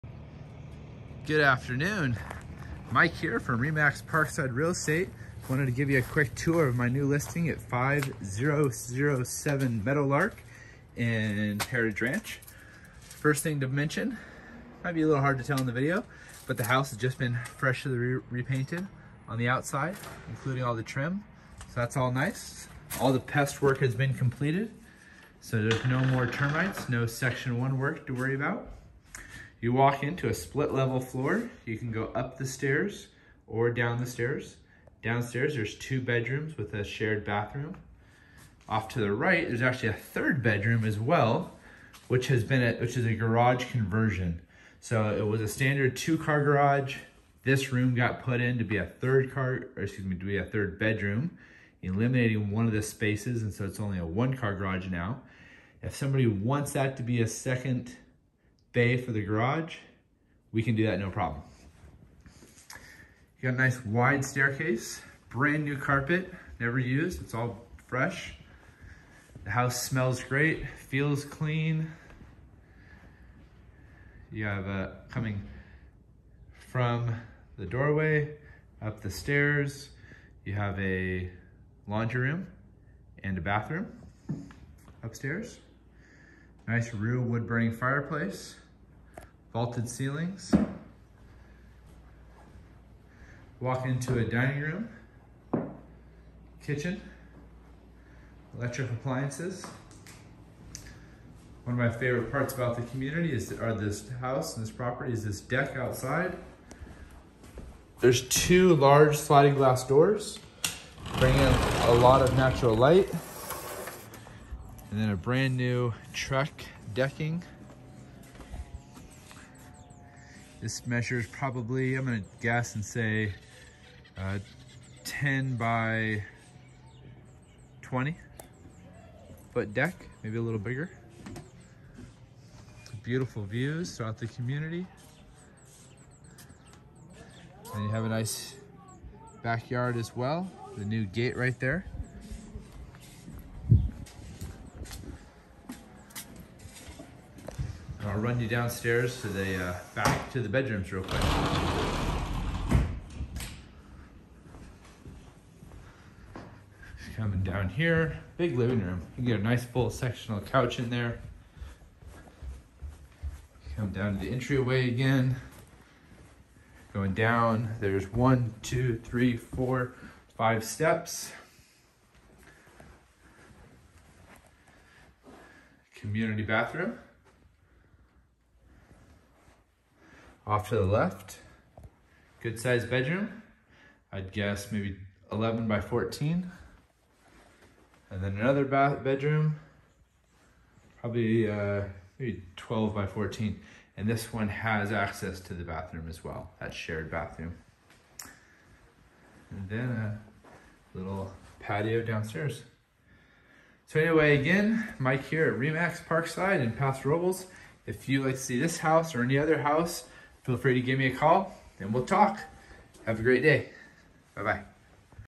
Available Now- 5007 Meadow Lark, Paso Robles -
Here is a preview of 5007 Meadow Lark at Heritage Ranch in Paso Robles. This property is move in ready and is going to be a great home for anyone to live. This 2bed 2bath with a bonus room is 1529 sqft and built in 1980. The next owner will love the fresh carpet and huge outside deck.